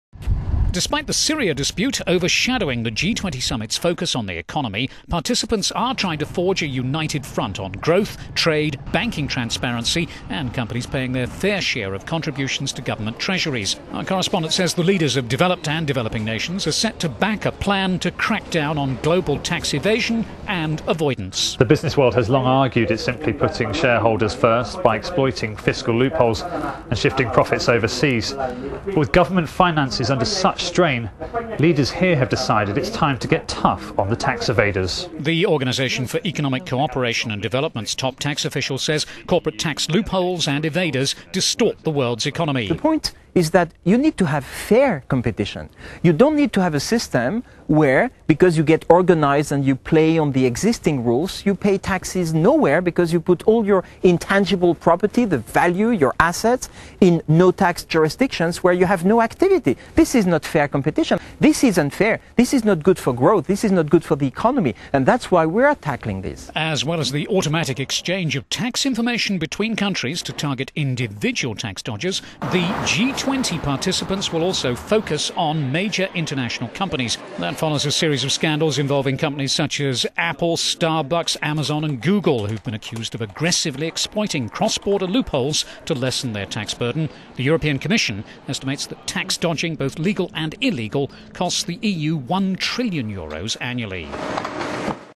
G20 focuses on tax dodging - economy
Despite the Syria dispute overshadowing the G20 Summit's focus on the economy, participants are...

Despite the Syria dispute overshadowing the G20 Summit's focus on the economy, participants are trying to forge a united front on growth, trade, banking transparency and tax - getting companies to pay their fair share of contributions to governments' treasuries.

The leaders of developed and developing nations are set to back a plan to crackdown on global tax evasion and avoidance.

James Franey. the euronews correspondent at the gathering at  St Petersburg in Russia said: "The business world has long argued that it is simply putting shareholders first by exploiting fiscal loopholes and shifting profits overseas. But with government finances under such strain, leaders here have decided it's time to get tough on the tax evaders."

The Organisation for Economic Cooperation and Development's top tax official Pascal Saint-Amans, told us corporate tax loopholes distort the world's economy: "The point is that you need to have fair competition. You don't need to have a system where because you get organised and you play on the existing rules you pay taxes nowhere because that you put all your intangible property, the value, your assets in no-tax jurisdiction where you have no activity. This is not fair competition. This is unfair. This is not good for growth and this is not good for the economy and that's why we are tackling this."

Saint-Amans told euronews that there is a "consensus amongst the G20 to take very significant steps" towards automatic exchange  of information to target individual tax dodgers and stop companies moving their profits overseas to avoid tax.

"People cannot accept to pay more taxes when some don't," he said, adding that an automatic exchange deal is "the end of bank secrecy".

The G20's focus on this issue follows a series of scandals involving companies - such as Apple, Amazon, Starbucks and Google - who have been accused of aggressively exploiting cross-border loopholes to lessen their tax burden.